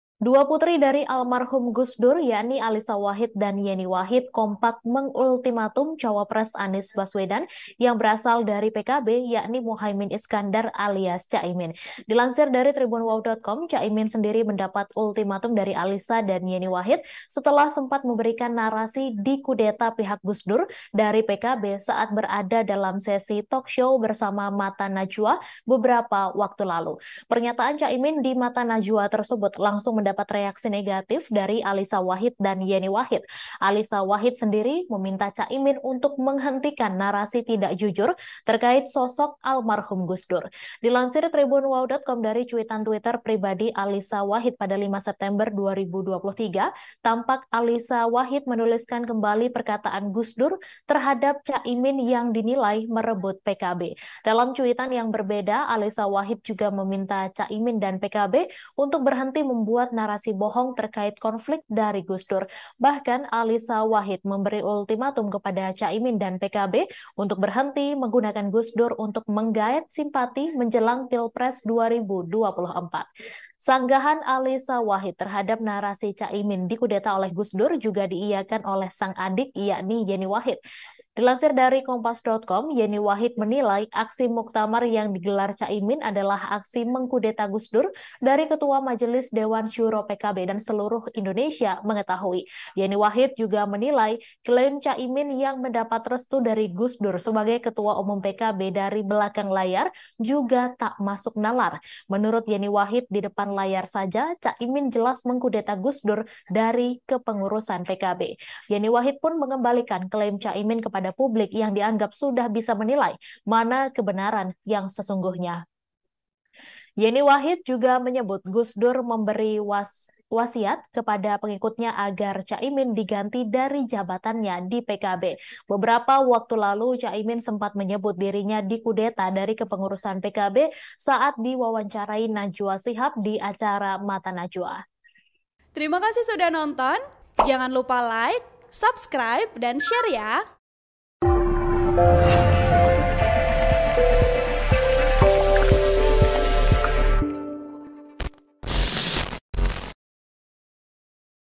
Alissa dan Yenny Wahid Ultimatum Cak Imin, Minta Berhenti 'Jualan Gus Dur' untuk Cari Dukungan
TRIBUN-VIDEO.COM - Dua putri dari almarhum Gus Dur, yakni Alissa Wahid dan Yenny Wahid kompak mengultimatum cawapres Anies Baswedan yang berasal dari PKB, yakni Muhaimin Iskandar alias Cak Imin.

Dilansir TribunWow.com, Cak Imin sendiri mendapat ultimatum dari Alissa Wahid dan Yenny Wahid setelah sempat memberikan narasi dikudeta pihak Gus Dur dari PKB saat berada dalam sesi talkshow bersama Mata Najwa beberapa waktu lalu.

Pernyataan Cak Imin di Mata Najwa tersebut langsung mendapat reaksi negatif dari Alissa Wahid dan Yenny Wahid.

Alissa Wahid sendiri meminta Cak Imin untuk menghentikan narasi tidak jujur terkait sosok almarhum Gus Dur.

Dilansir TribunWow.com dari cuitan Twitter pribadi @AlissaWahid pada 5 September 2023, tampak Alissa Wahid menuliskan kembali perkataan Gus Dur terhadap Cak Imin yang dinilai merebut PKB.

"Saya memang tidak pernah terlibat masuk di PKB. Tapi saya jelas mengingat betul ucapan GusDur LANGSUNG kepada saya : "Imin merebut PKB dan tidak bisa dibiarkan". Sudahlah hentikan narasi tidak jujur seperti ini," cuit Alissa Wahid.

Dalam cuitan yang berbeda, Alissa Wahid juga meminta Cak Imin dan PKB untuk berhenti membuat narasi bohong terkait konflik dari Gus Dur.

Bahkan, Alissa Wahid memberi ultimatum kepada Cak Imin dan PKB untuk berhenti menggunakan Gus Dur untuk menggaet simpati jelang Pilpres 2024 nanti.

"Permintaan saya cuma ini : PKB berhenti membuat narasi bohong tentang konflik GusDur & Imin. Akui, dulu menyakiti GD. Saya saksi hidup bagaimana dampak hal itu pd kesehatan beliau. Stop jualan GusDur buat mencari dukungan publik. Toh sudah merasa sukses. Sudah, itu saja!," lanjut Alissa Wahid.

Sanggahan Alissa Wahid terhadap narasi Cak Imin dikudeta oleh Gus Dur juga diiyakan oleh sang adik, yakni Yenny Wahid.

Dilansir TribunWow.com dari Kompas.com pada Selasa, 5 Agustus 2023, Yenny Wahid menilai aksi muktamar yang digelar Cak Imin adalah aksi mengkudeta Gus Dur dari Ketua Majelis Dewan Syuro PKB dan seluruh Indonesia tahu.

"Jadi muktamar Ancol itu ganti Gus Dur dari Ketua Dewan Syuro, dan itu proses yang diketahui oleh publik Indonesia. Dan itu jelas sekali dari awal menjadi problem besar bagi kami karena Gus Dur dilengserkan dari Ketua Umum Dewan Syuro," tutur Yenny Wahid.

Yenny Wahid juga menilai klaim Cak Imin yang mendapat restu dari Gus Dur sebagai Ketua Umum PKB dari belakang layar juga tak masuk nalar.

Menurut Yenny Wahid, di depan layar saja Cak Imin jelas mengkudeta Gus Dur dari kepengurusan PKB.

"Muktamar Ancol kurang apa terang benderangnya. Di situ Gus Dur diganti, di situ Gus Dur dikudeta. Kok masih klaim menyatakan sebaliknya," lanjut Yenny Wahid.

Yenny Wahid pun mengembalikan klaim Cak Imin kepada publik yang dianggap sudah bisa menilai mana kebenaran yang sesungguhnya.

Yenny Wahid juga menyebut Gus Dur memberi wasitan kepada pengikutnya agar Cak Imin diganti dari jabatannya di PKB.

"Jadi saya rasa bukti-bukti formal ya menunjukkan bahwa memang telah terjadi pengkudetaan terhadap Gus Dur. Cak imin boleh saja mengklaim apapun, boleh saja. Tapi sampai beliau wafat, Bapak memang masih berwasiat Cak Imin harus diganti," tambah Yenny Wahid.

Beberapa waktu lalu, Cak Imin sempat menyebut dirinya dikudeta dari kepengurusan PKB saat diwawancarai Najwa Shihab di acara Mata Najwa.

"Jangan dibalik-balik saya mengkudeta Gus Dur, saya yang dikudeta, tetapi saya terima. Itulah yang terjadi," ujar Cak Imin.(*)

Artikel ini telah tayang di TribunWow.com dengan judul Putri Gus Dur, Alissa dan Yenny Wahid Ultimatum Cak Imin, Minta Hentikan Klaim Narasi Dikudeta,